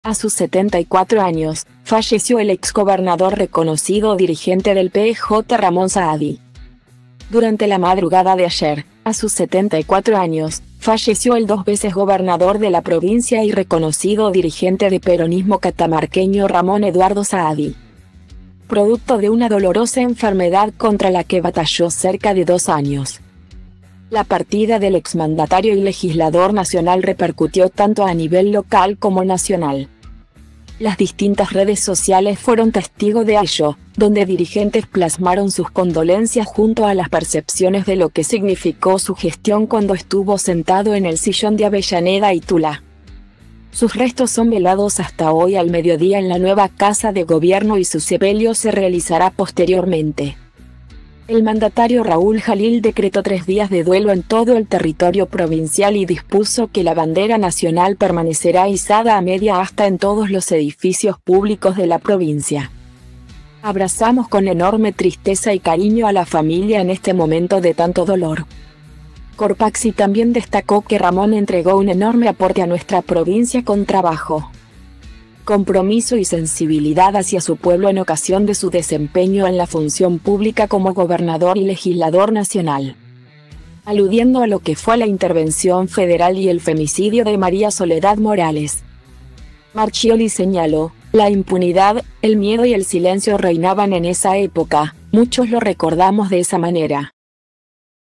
A sus 74 años, falleció el exgobernador reconocido dirigente del PJ Ramón Saadi
Durante la madrugada de ayer, a sus 74 años, falleció el dos veces gobernador de la provincia y reconocido dirigente de peronismo catamarqueño Ramón Eduardo Saadi,producto de una dolorosa enfermedad contra la que batalló cerca de dos años. La partida del exmandatario y legislador nacional repercutió tanto a nivel local como nacional.